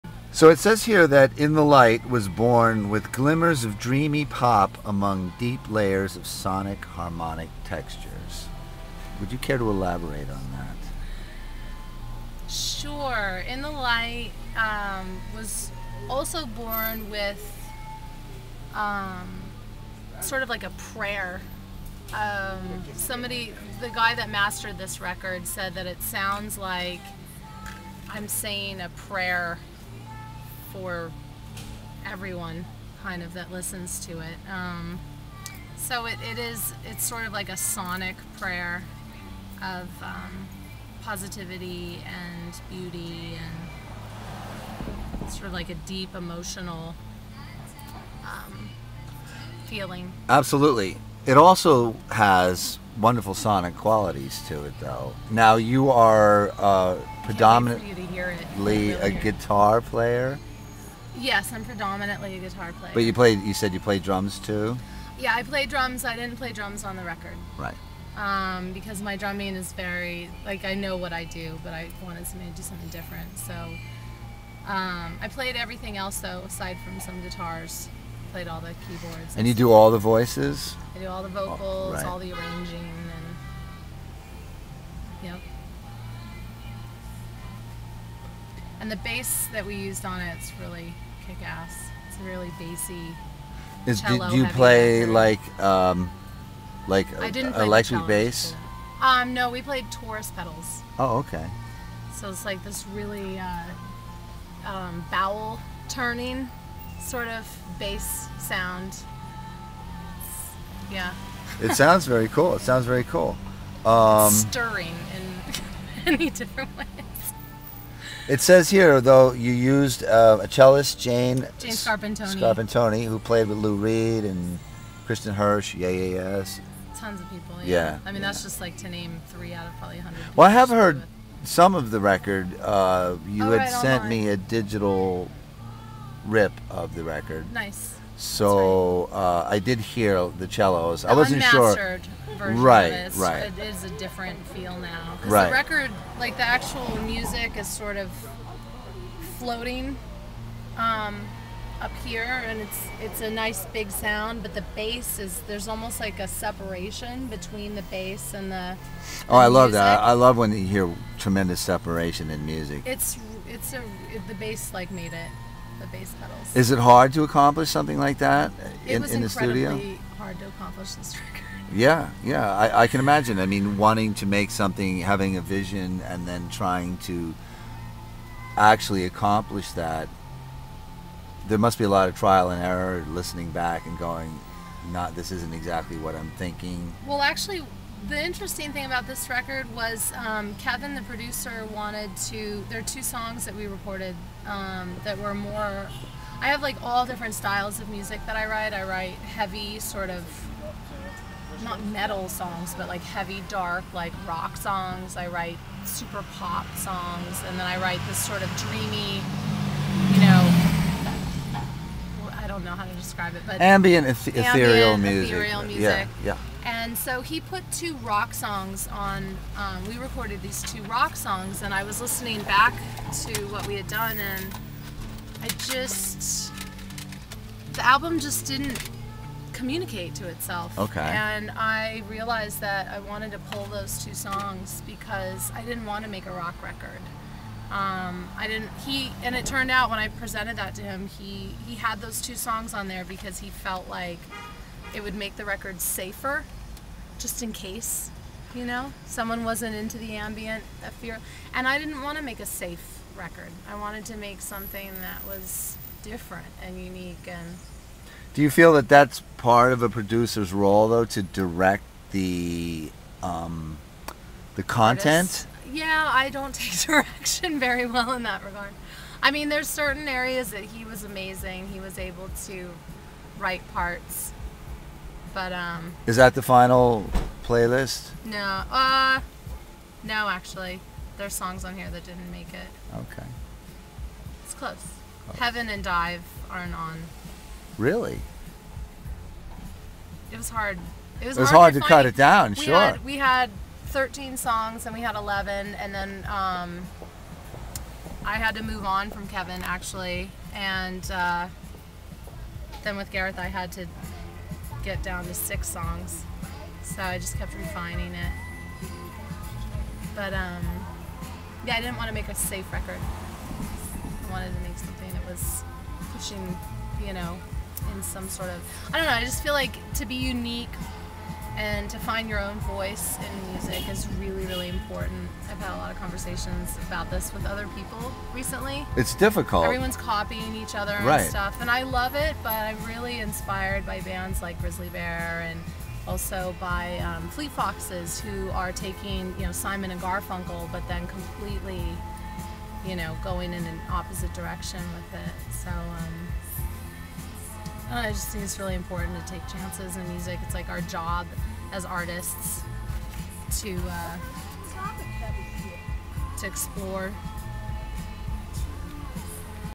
Shana Falana Interview - Part 1
Dave Cromwell interviews musician Shana Falana on Saturday September 10, 2011 for DaveCromwellWrites.blogspot.com